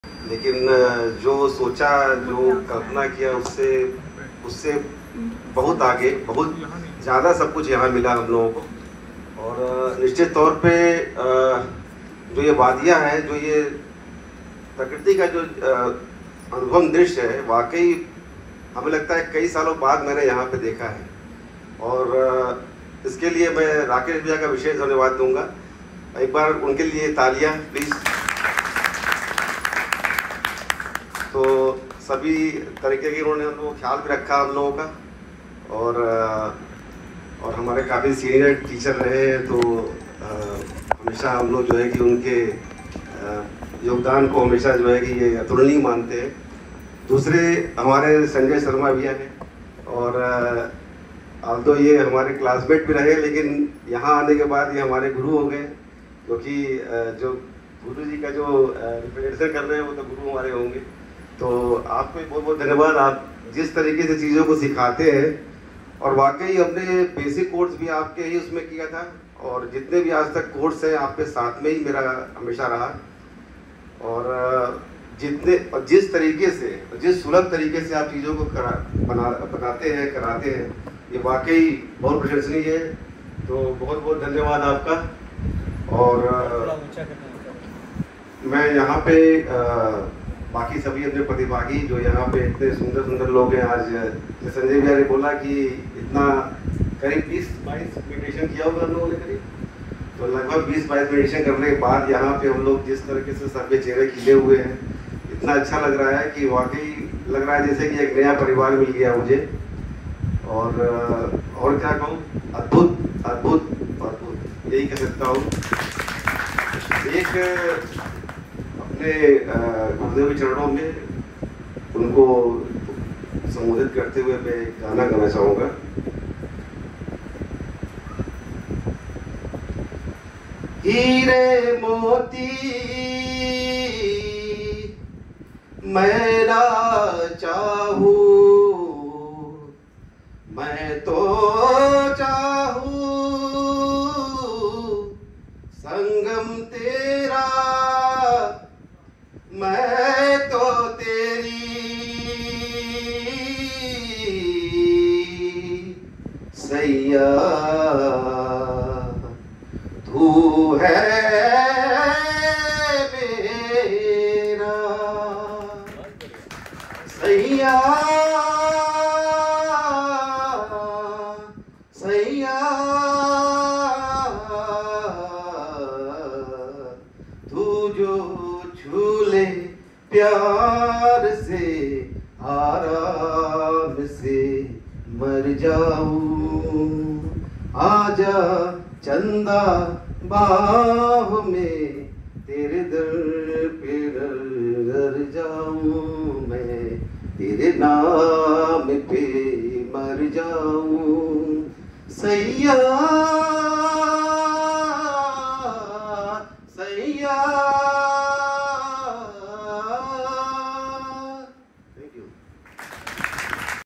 Transformative Experiences: Vinay ji Shares his Journey at the Advanced Meditation Program
Discover the profound impact of the advanced meditation programme held at Sri Sri Kannan under the expert guidance of Sanjay Sharma. In this inspiring video, participants open up about their personal journeys, sharing how the practices have deepened their meditation, enhanced their spiritual growth, and brought lasting peace and clarity to their lives. Hear firsthand accounts of the transformative power of meditation and how this retreat has touched the hearts and minds of those who attended.